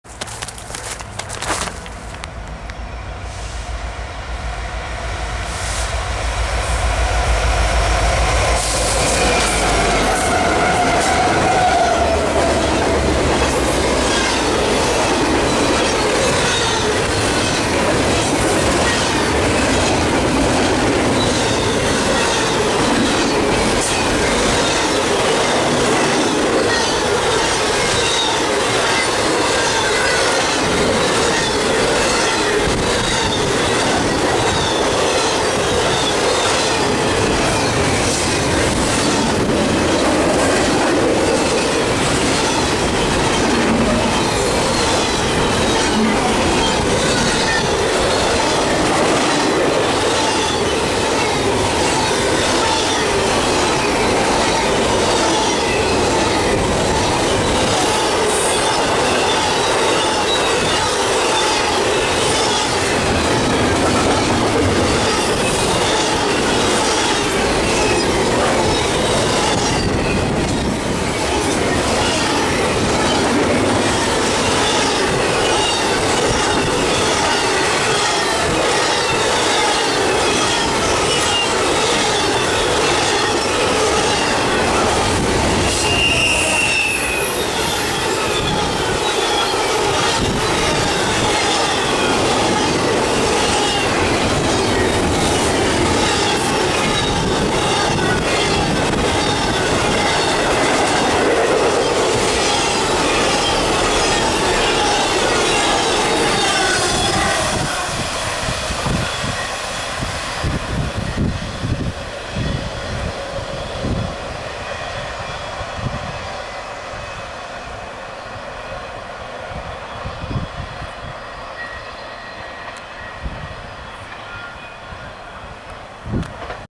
ACC leads NS 25A Flying thru Duluth, GA 6/22/2024
Power: NS 1807, NS 4156, NS 8132
Speed: 58 mph
Location: Near MP 610, Duluth, GA